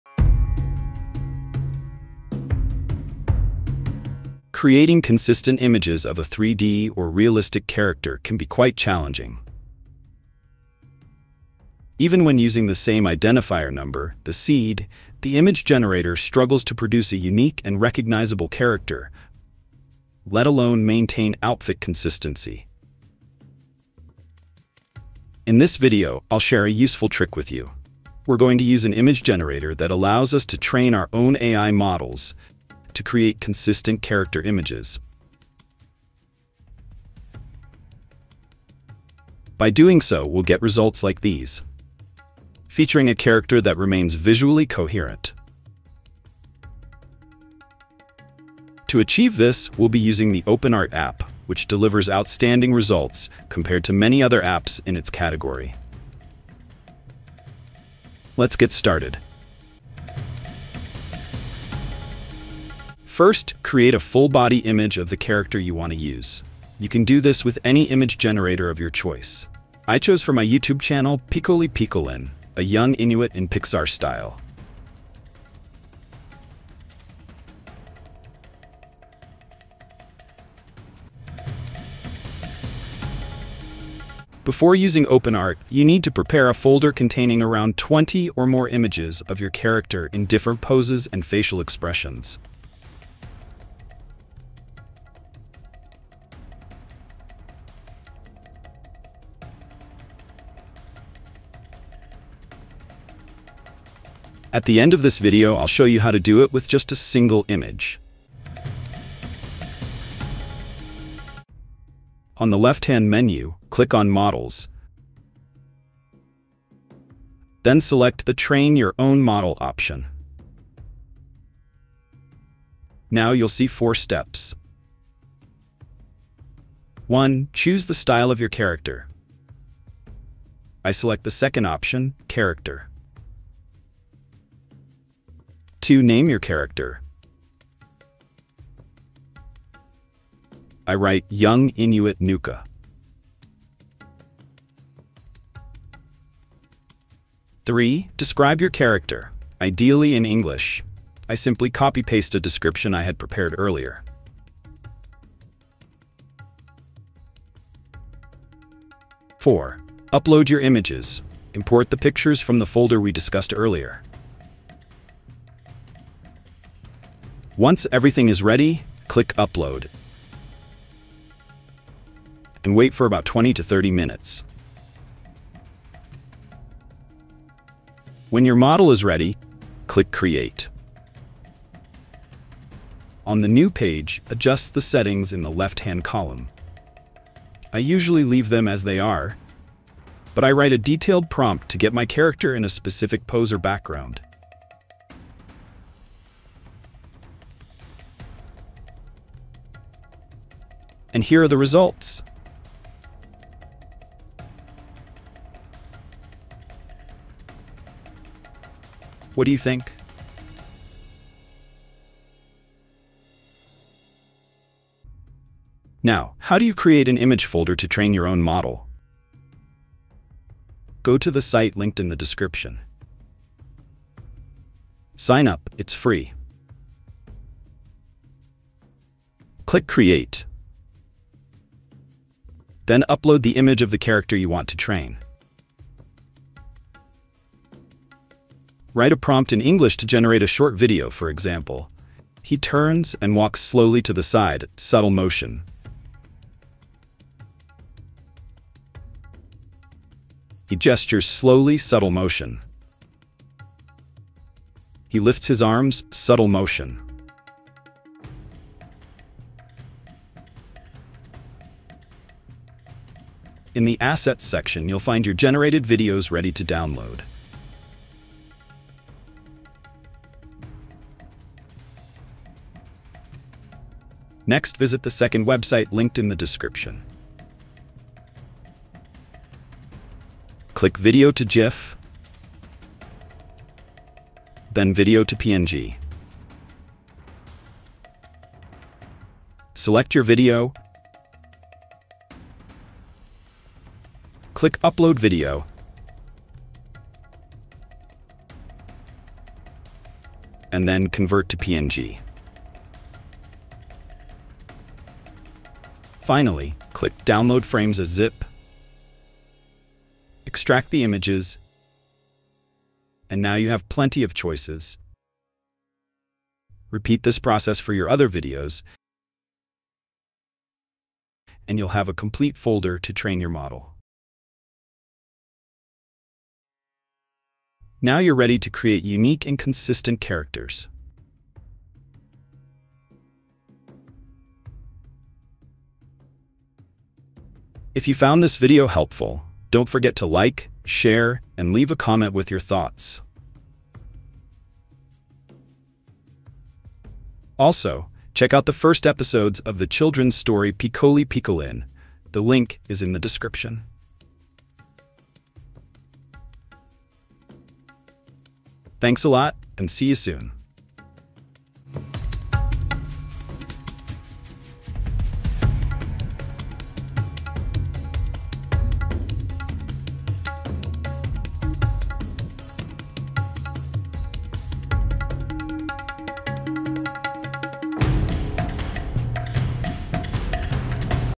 How to Create Consistent Characters with AI: OpenArt Trick !
🎨 Struggling to create consistent characters with AI? Even when using the same seed ID, your image generator just can’t keep the model consistent? 😩 Don’t worry! In this video, I’ll show you how to train your own AI model to generate 3D or realistic characters that are unique and consistent, with their outfits and expressions perfectly intact! 👌✨

🚀 In this tutorial, you’ll learn:
1️⃣ How to use OpenArt to generate consistent images.
2️⃣ How to create an image folder to train your AI model with different poses and facial expressions.
3️⃣ How to convert videos into images (PNG) to easily enrich your model.
4️⃣ How to get Pixar-style characters, like my character Picoli Picolin, a young Inuit I created for my channel! 🐧❄️

🔧 Tools used:

OpenArt for training custom AI models
Video-to-GIF and Video-to-PNG converters for extracting frames
AI image generators for the initial visuals
🔥 The result? You’ll have consistent characters in every image, perfect for creative projects, videos, or even your own animated stories!